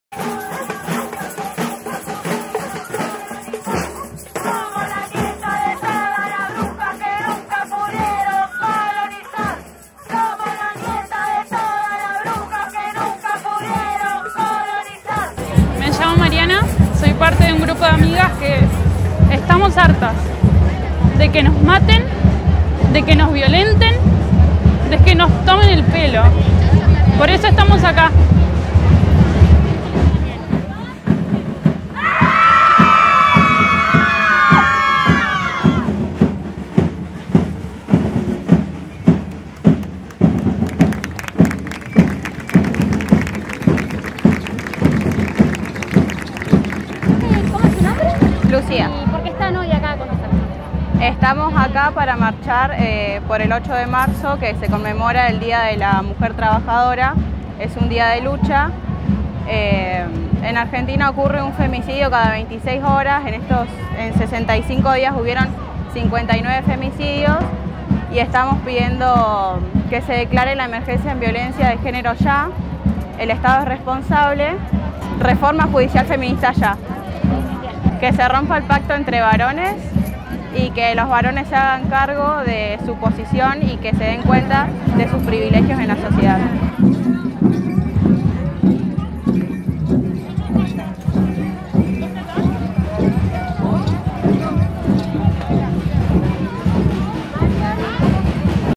🔥📢💜 PARO FEMINISTA FUEGO FEMINISTA JUSTICIA FEMINISTA
8Marzo2021​​ Paro Plurinacional de Mujeres, Lesbianas, Travestis, Trans y No Binaries. 🔥📢💜

Imágenes Ayelén Santillan